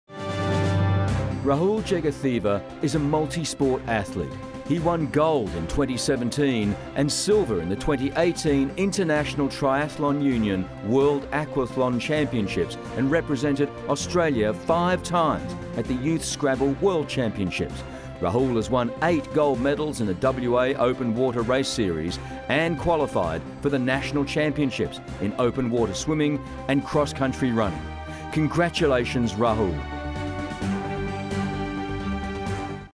2019 WA Young Achiever Awards SPORTS FINALIST Rahul Jegatheva
Rahul Jegatheva - Finalist for the Sports Award at the 2019 WA Young Achievers Awards.

FINALISTS
Brianna Throssel - Olympian (Winner)
Rhiannon Clarke - Paralympian
Mark Daniels - Paralympian
Rahul Jegatheva - World Champion